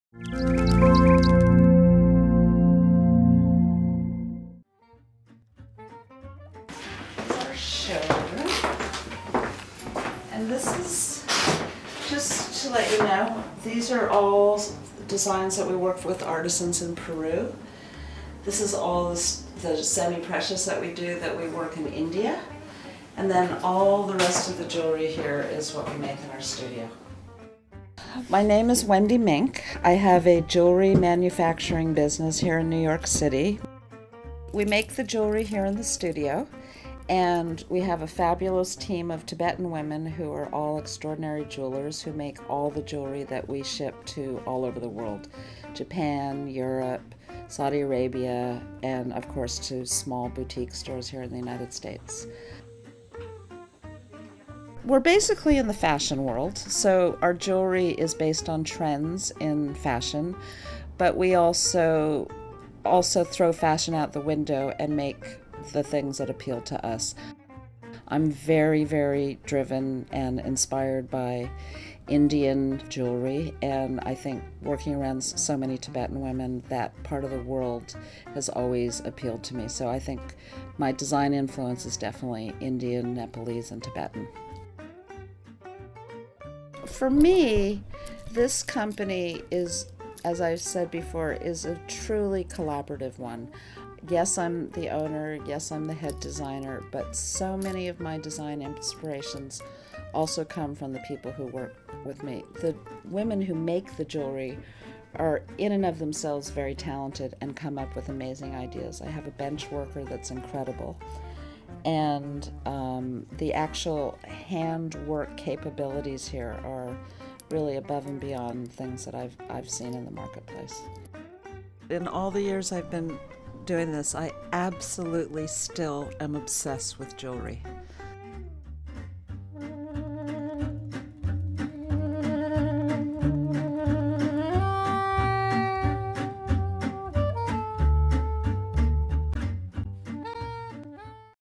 The Artisan Bazaar: Wendy Mink Jewelry Studio
The Wendy Mink Jewelry studio is located in NYC's Tribeca neighborhood where a team of Tibetan jewelry artisans work right alongside production sales staff.

In March 2012, Wendy will be participating in a pop-up event (theartisanbazaarnyc.com) at the Chelsea Market in NYC.